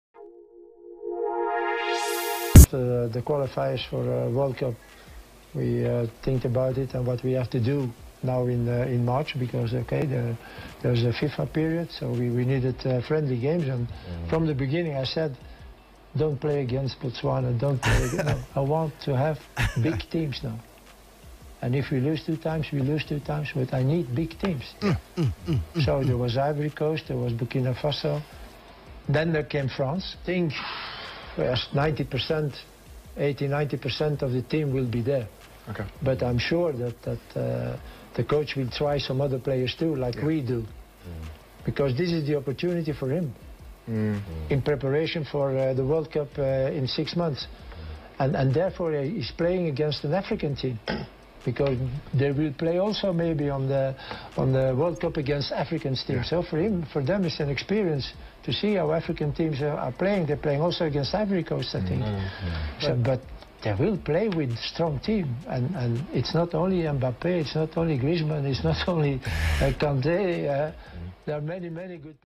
Hugo Broos - I need big teams not Botswana
Bafana Bafana coach Hugo Broos says there was Ivory Coast on the list but choosen France. He said ,he told SAFA not to bring Botswana he wanted big teams.